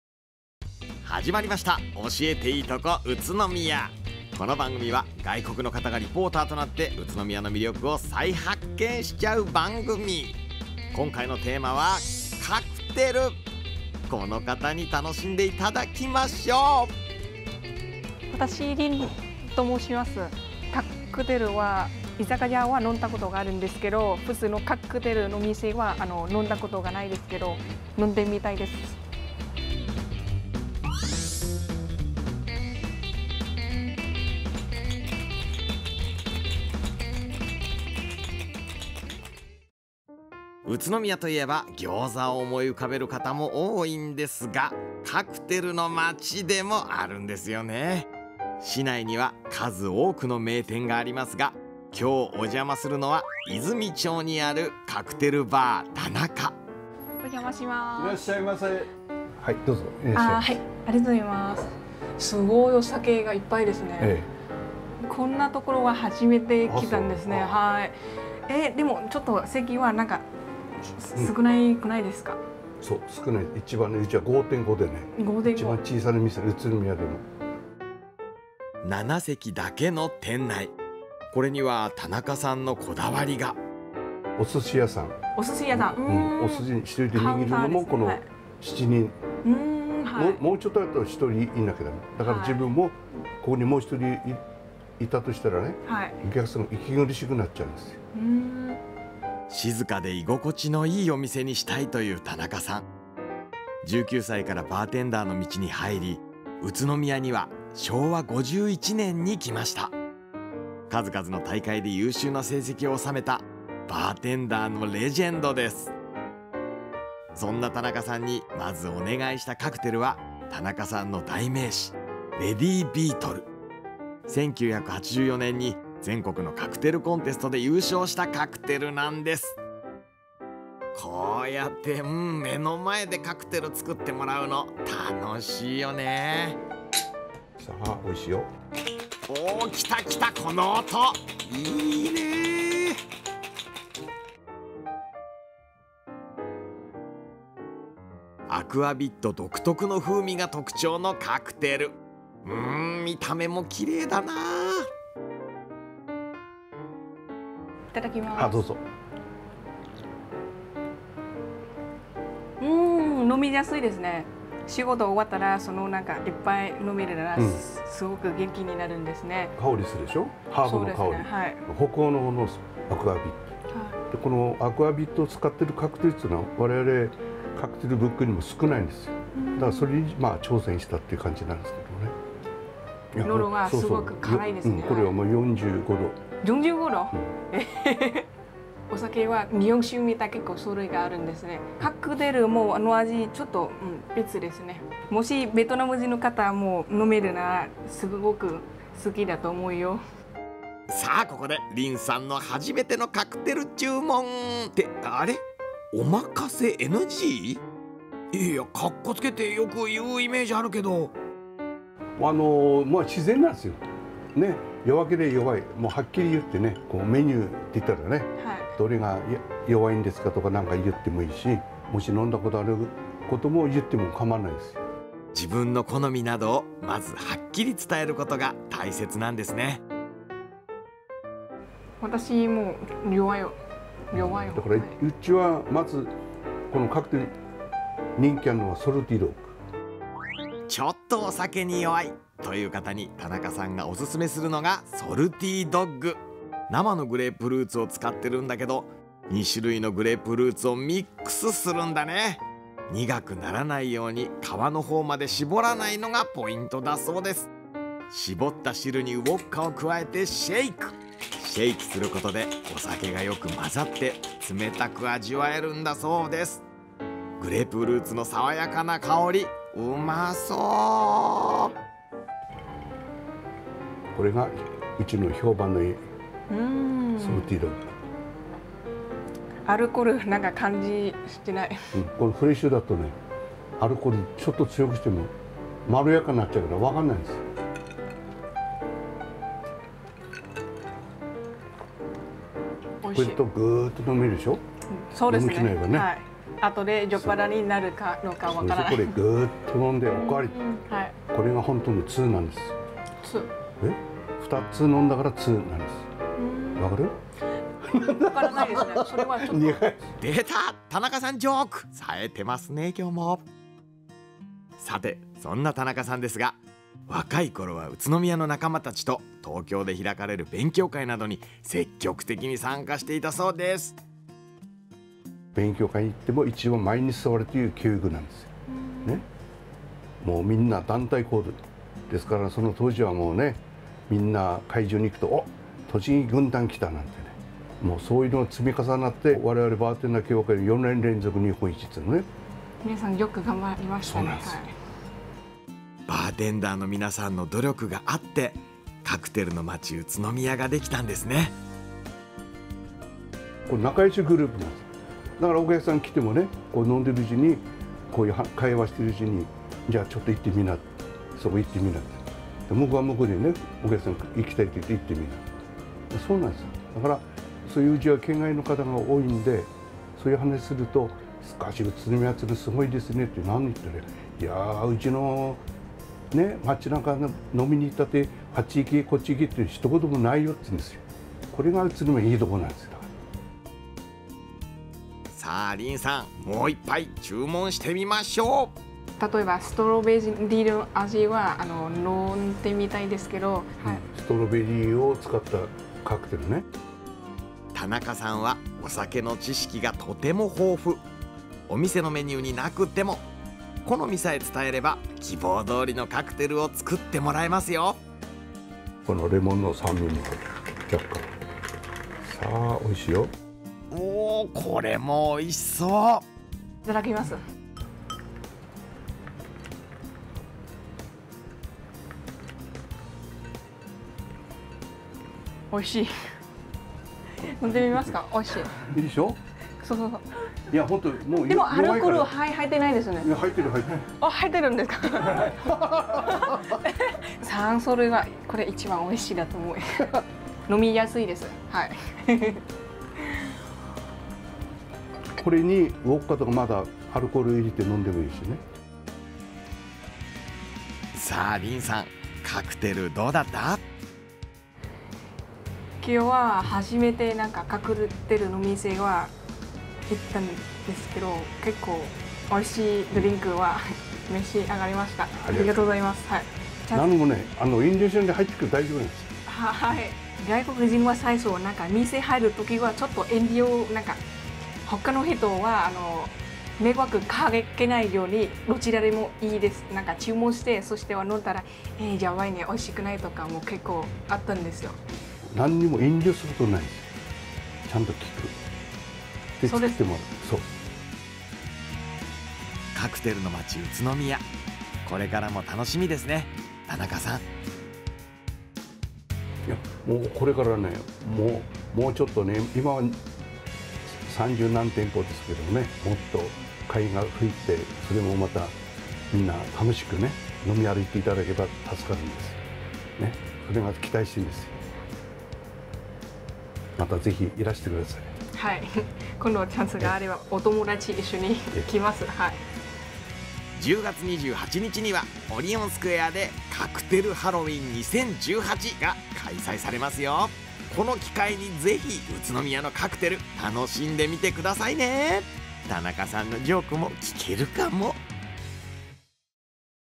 【宇都宮市】教えてイイトコUTSUNOMIYA（カクテル）
今回は、カクテルのまち宇都宮の名店の一つで、泉町にある「カクテルバータナカ」に行きました。